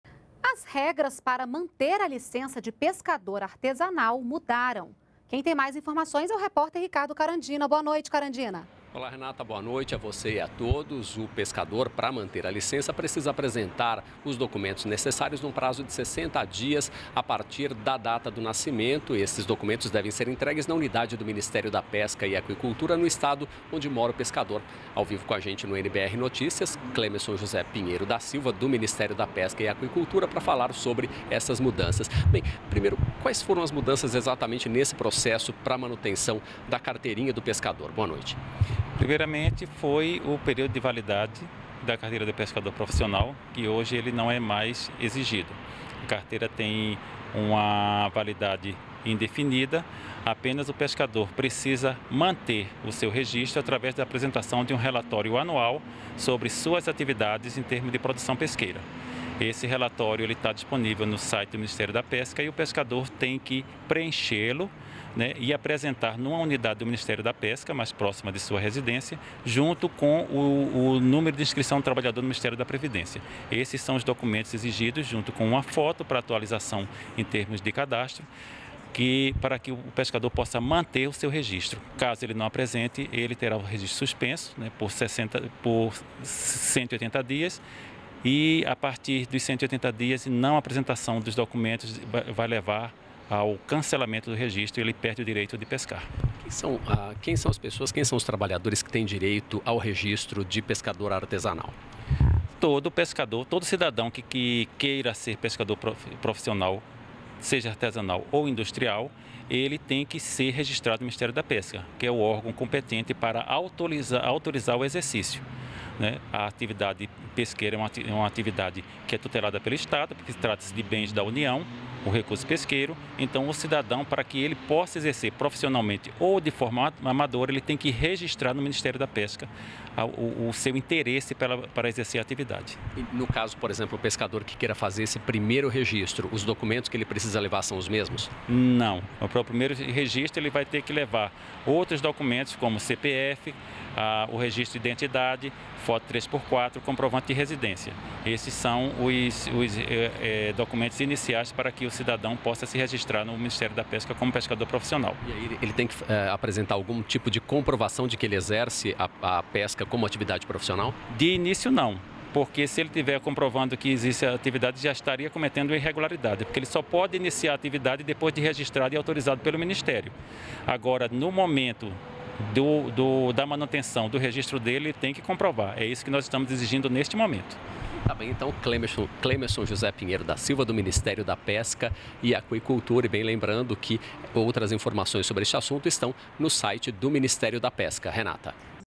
Manutenção da Carteira de Pescador Artesanal tem novas regras
NBR NOTÍCIAS - 13.08.14: A licença de pescador artesanal não terá mais prazo de validade. Mas, para manter o registro, o trabalhador precisa apresentar um relatório anual sobre suas atividades de produção pesqueira, acompanhado do seu número de inscrição no Ministério da Previdência e de uma foto atual. Mais detalhes na entrevista com o diretor do Departamento de Registro da Pesca e Aquicultura, Clemeson José Pinheiro.